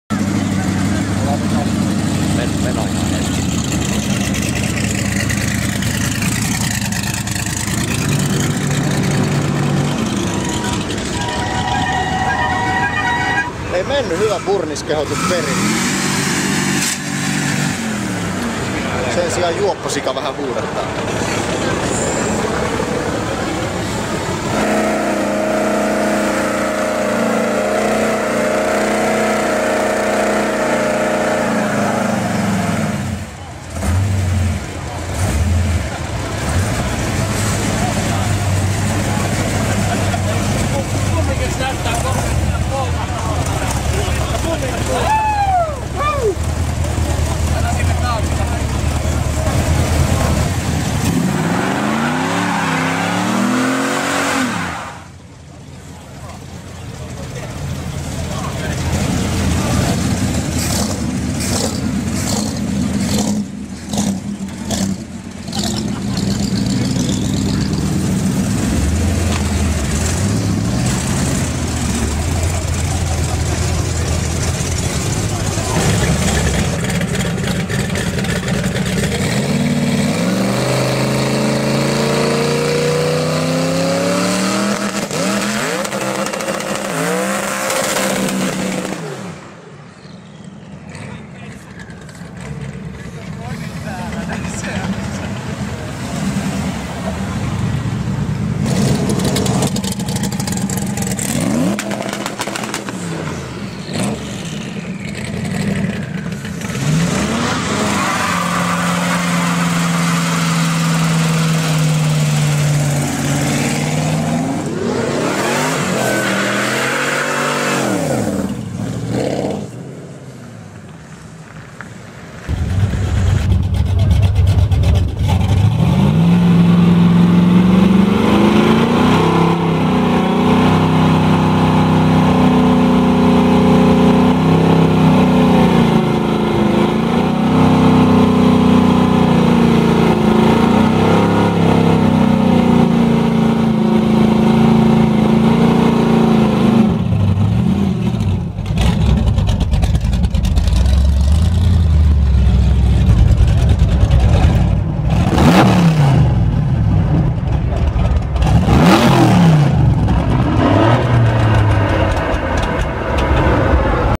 Lahti Cruising - Burnouts - May Day Eve 2019
A few burnouts at Lahti May Day Eve cruising. My video camera has lived its life, so I filmed these with an SLR camera. Planning to upgrade soon!